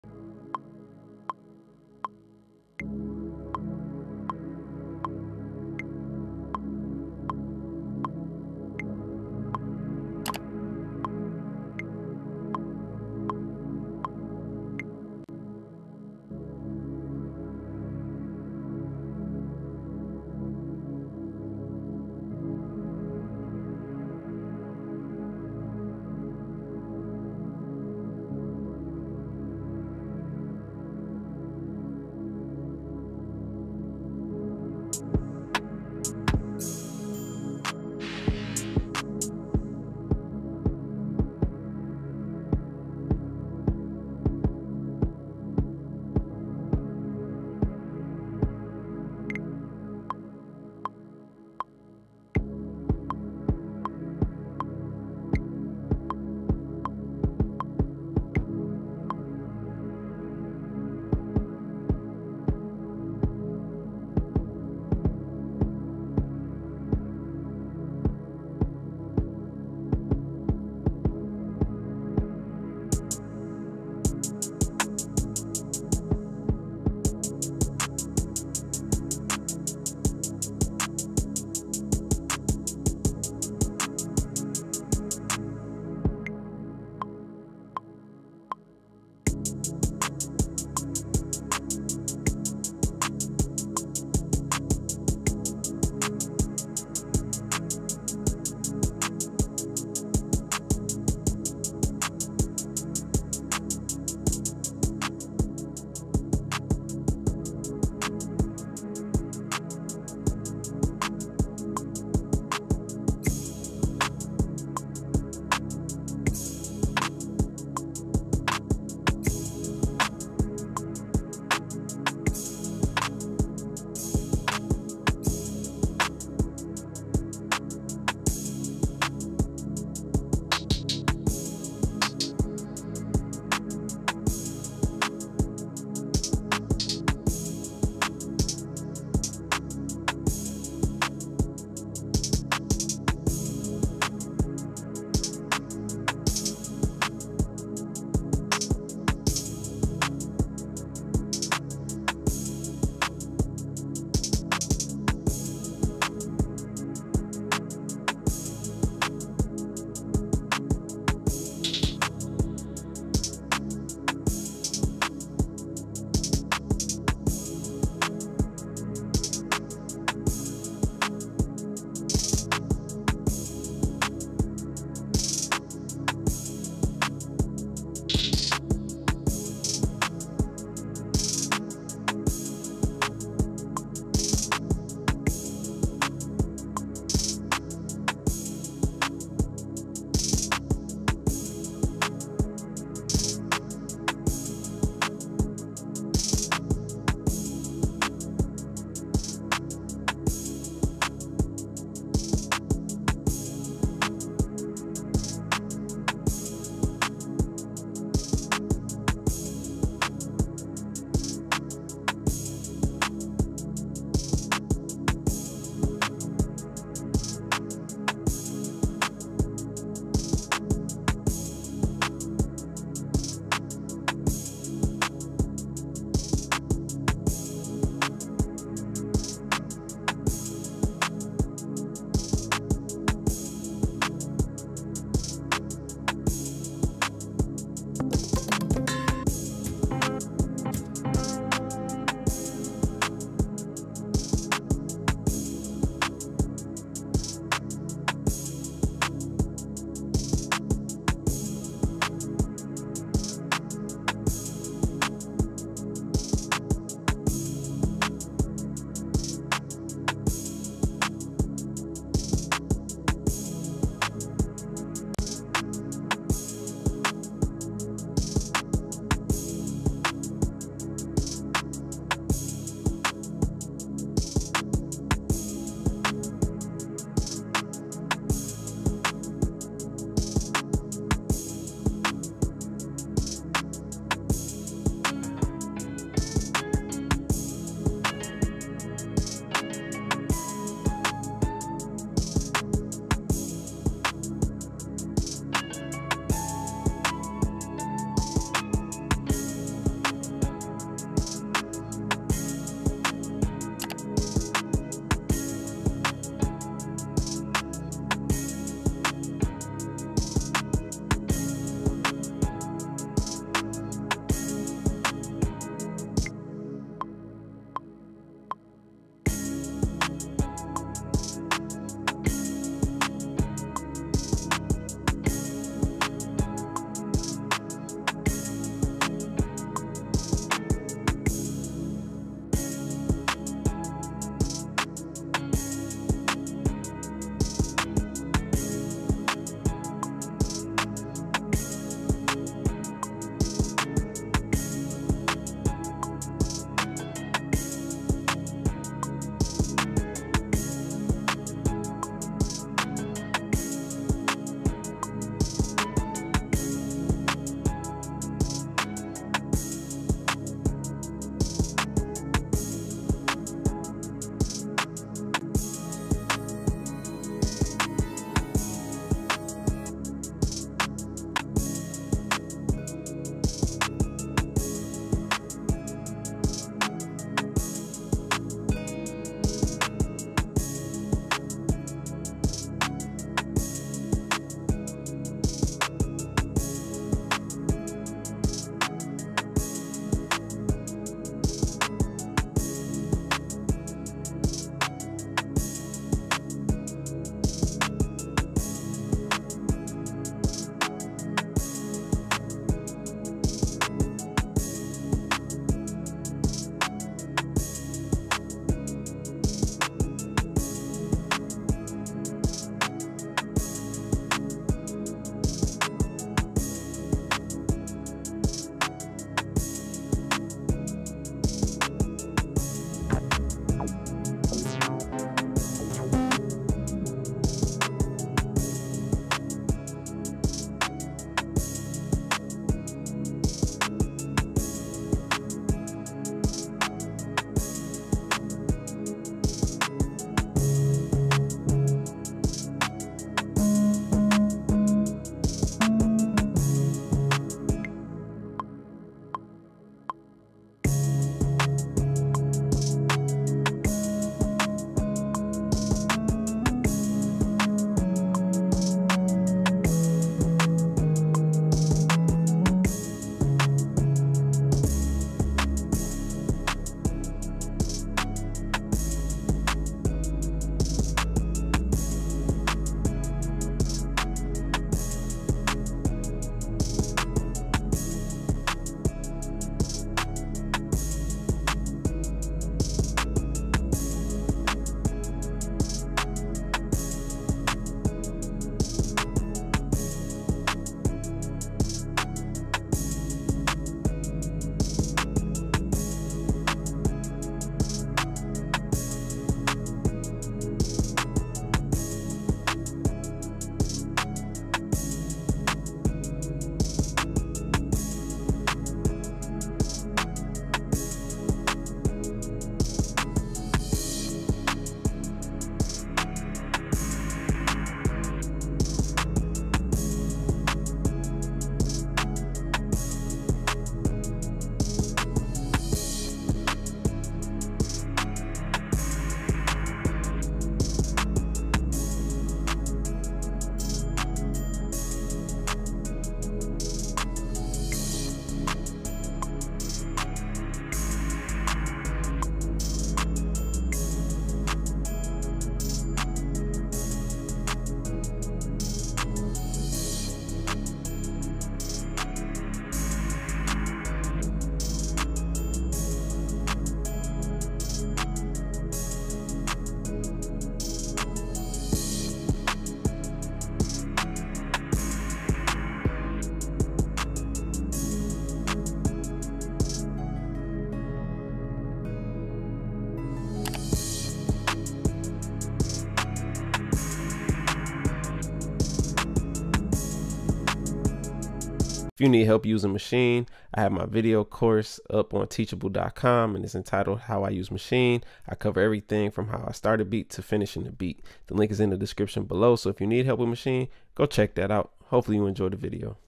I Made This Beat in Less Than 10 Minutes! Maschine Beat Making
Upload your music to streaming services such as Spotify, Apple Music & More

The Camera I use
The Mic
Studio Monitors
Audio Interface